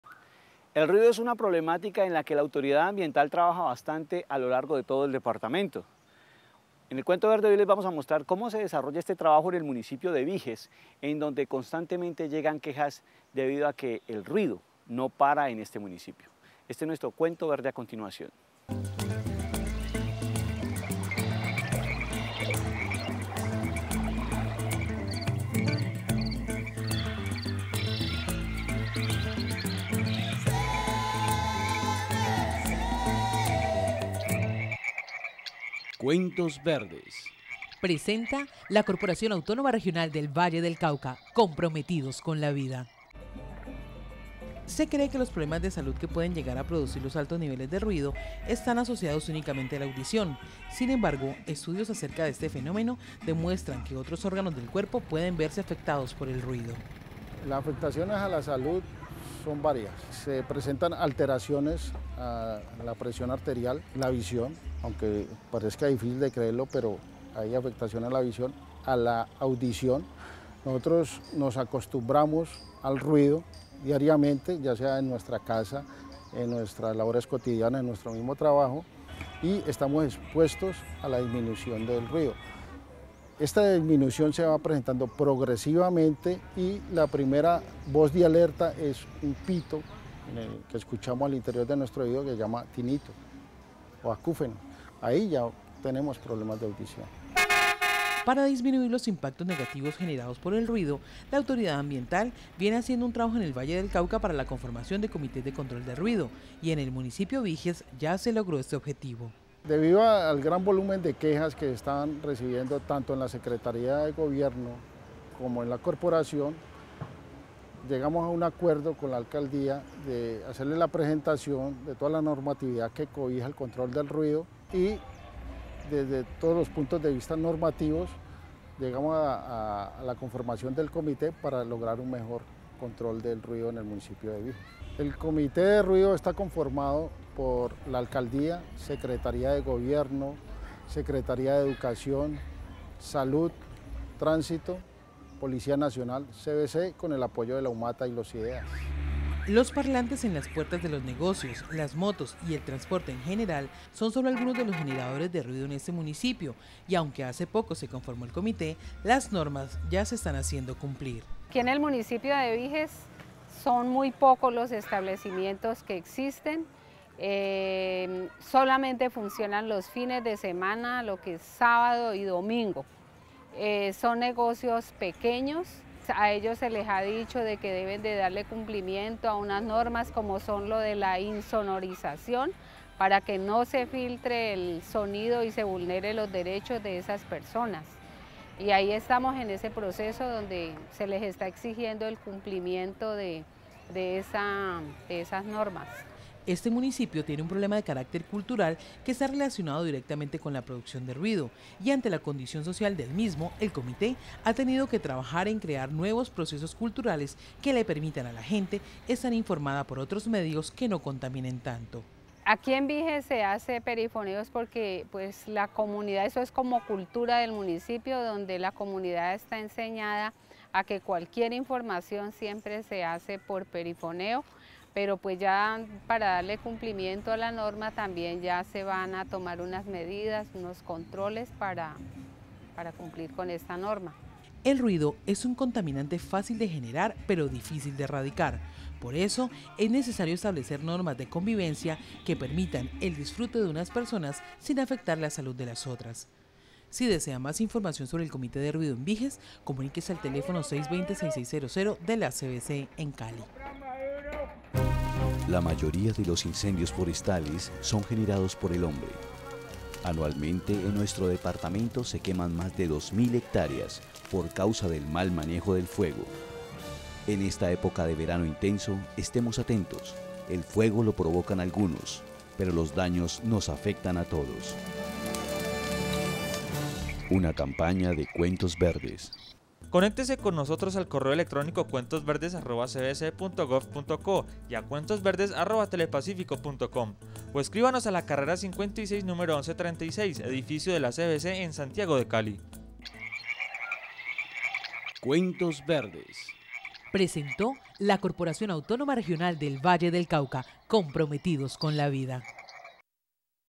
Cuentos Verdes Septiembre 10
RUIDO EN VIJES
El ruido es un problema en el que la autoridad ambiental trabaja constantemente, en el cuento verde de hoy conoceremos como se ha desarrollado este trabajo en el municipio Vijes, donde las quejas por el exceso de este fenómeno no paran, vea la historia completa hoy.